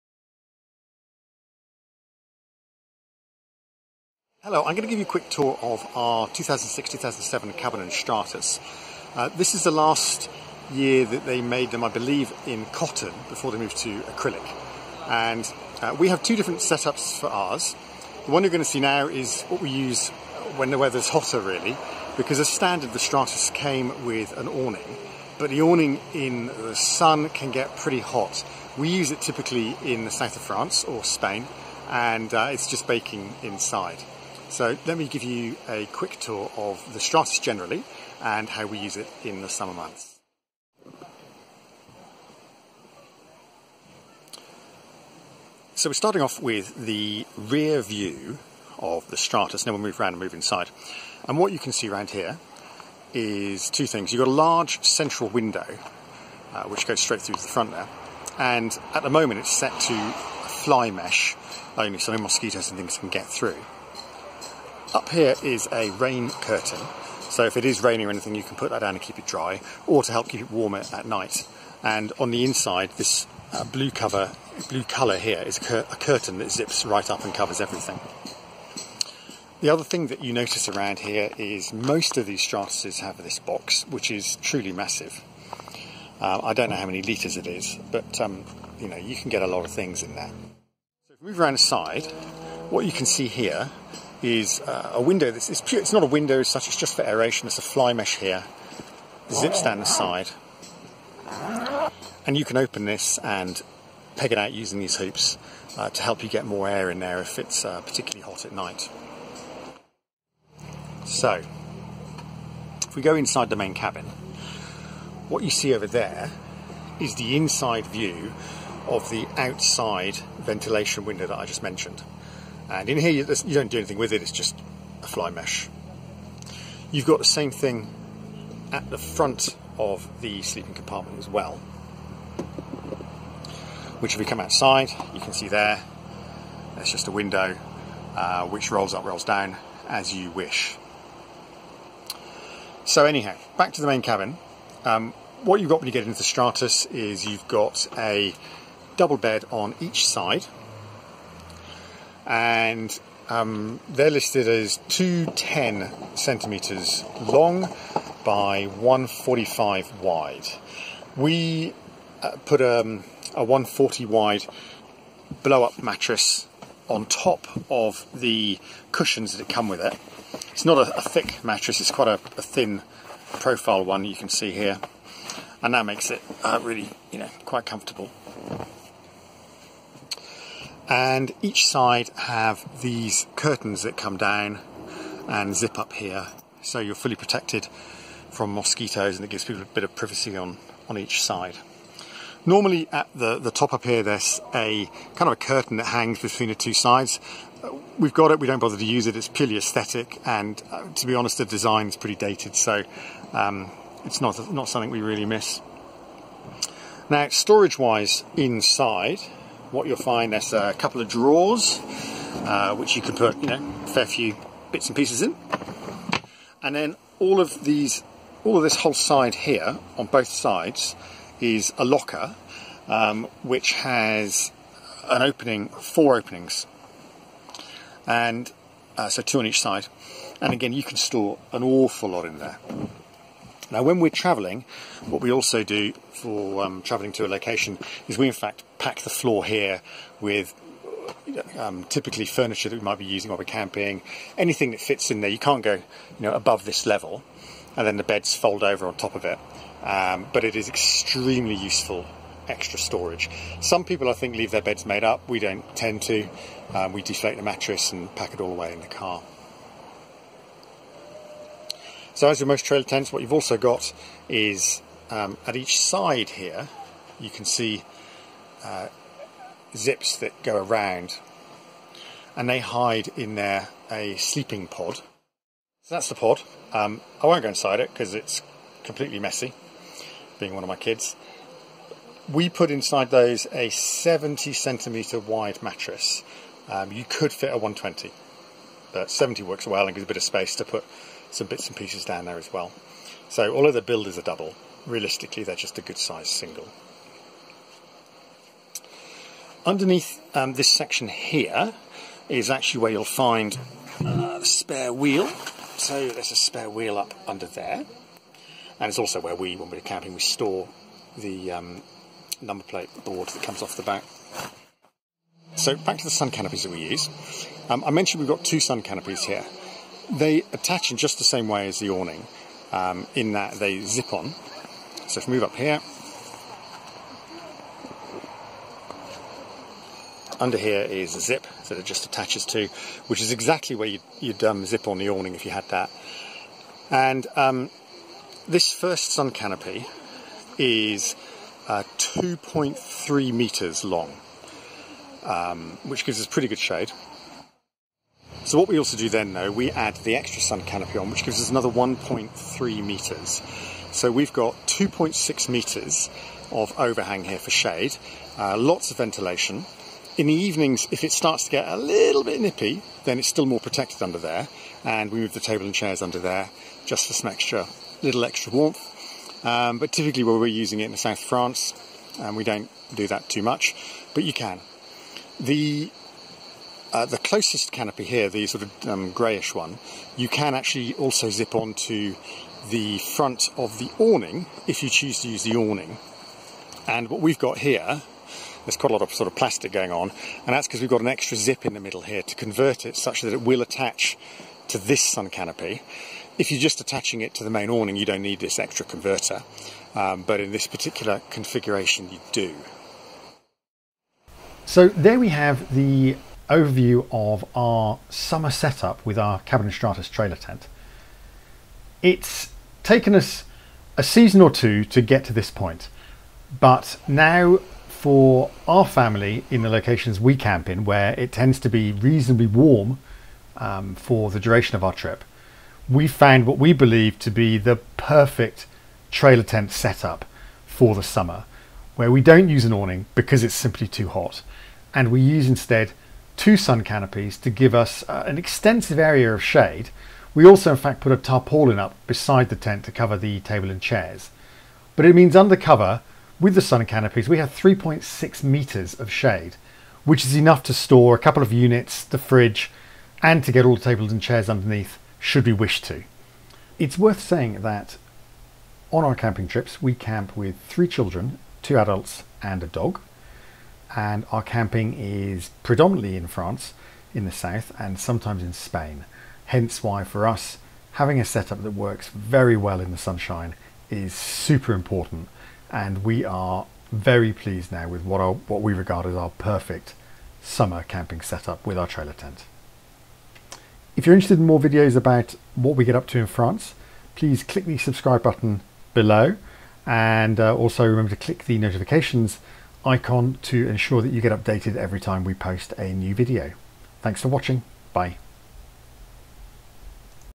Cabanon Stratos or Cabanon Mercury trailer tent - the best summer setup
The best trailer tent summer setup with a Cabanon Stratos or Mercury trailer tent. These are very similar trailer tents and the canopy solution we use will work on both. See how we use sun canopies rather than an awning during the summer.